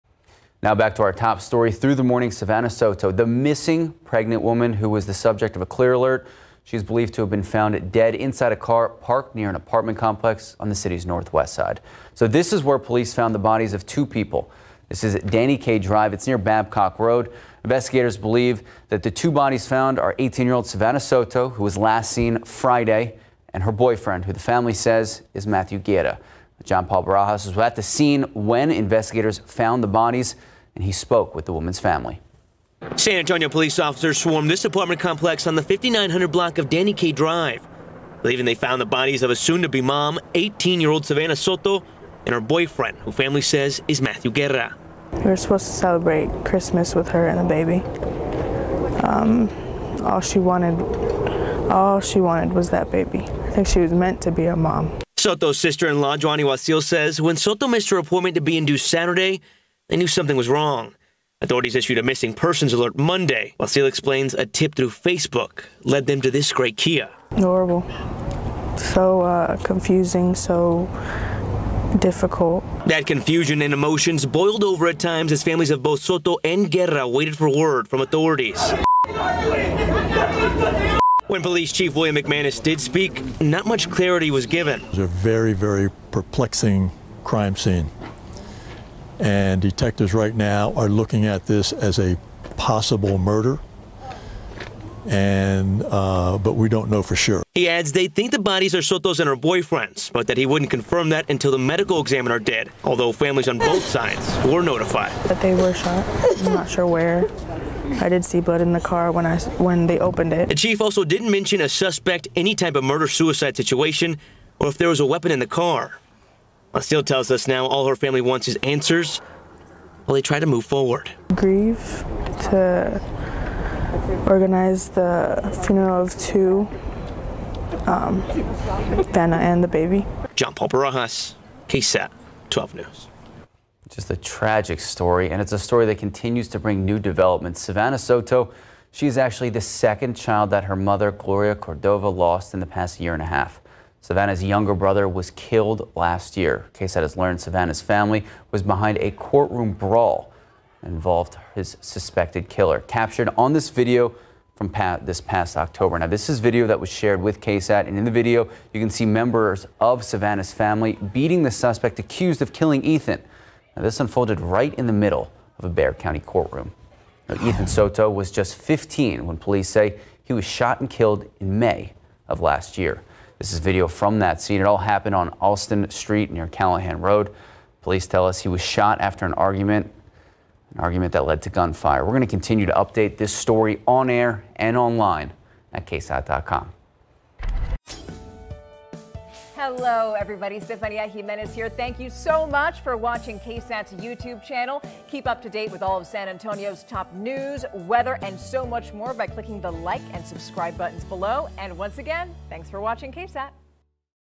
What we know about the case of Savanah Soto, the missing pregnant woman found dead in car with b...
The search for Savanah Soto, a pregnant woman who was reported missing by her family when she didn’t show up at the hospital to be induced, ended in tragedy on Tuesday.